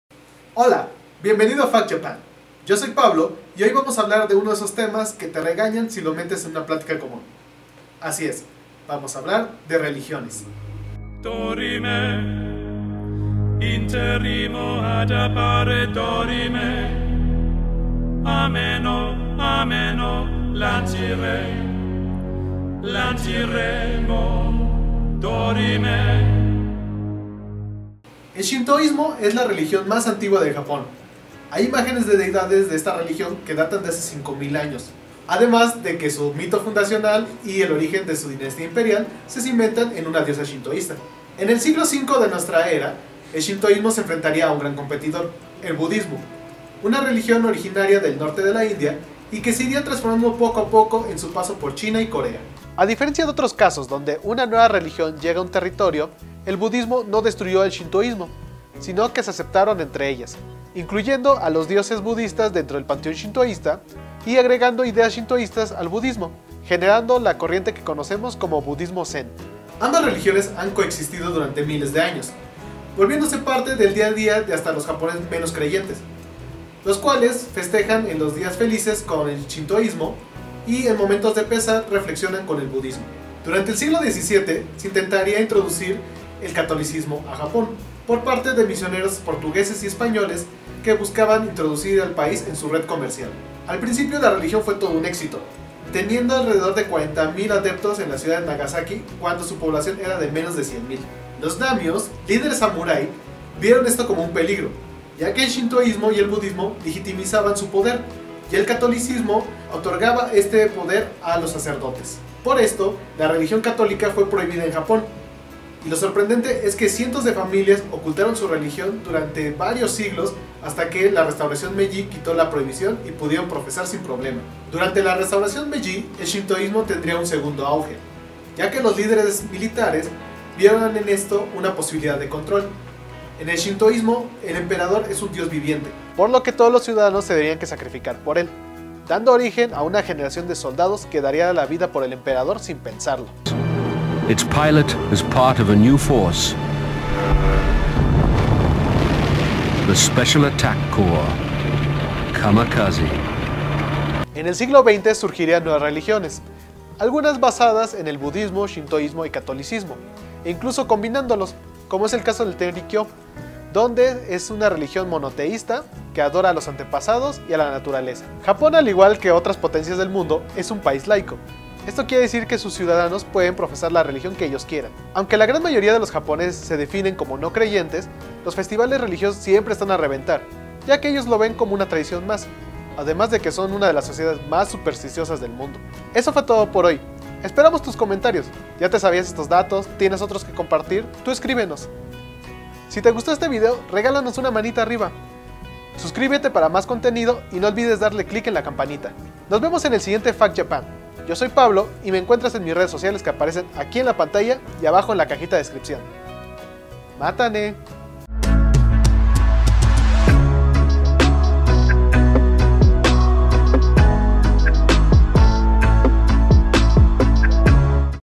¿Qué religión profesan en Japón?
¿Son los japoneses creyentes? ¿Cuáles son las religiones más populares en Japón? Ésta y otras preguntas nos resuelve Pablo en la edición de ésta semana de Fact Japan.